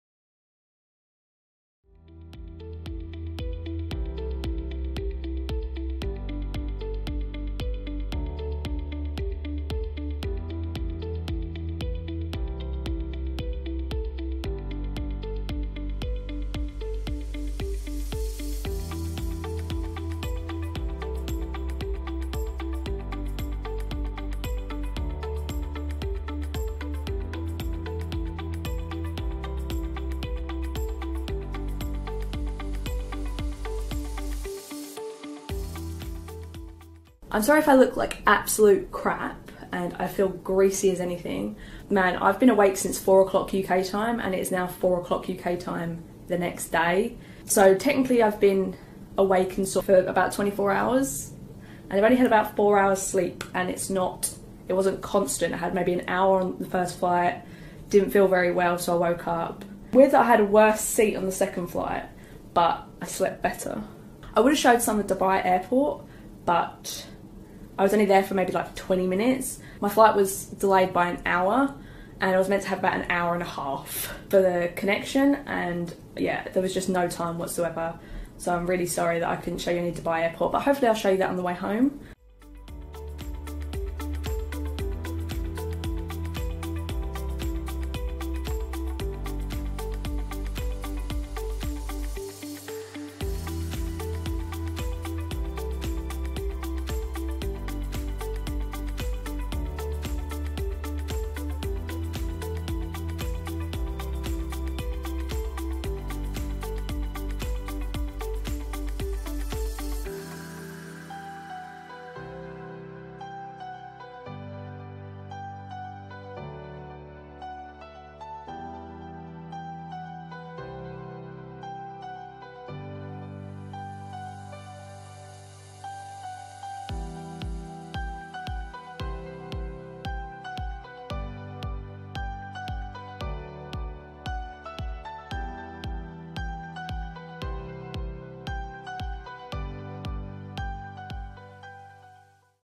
5 STAR HOTEL IN KUALA LUMPUR | Shangri La Room Tour & Mini Bar
Hi guys, this is my five star hotel that i am staying in for three weeks in Kuala Lumpur.

I am so grateful to be able to stay in this extraordinary hotel.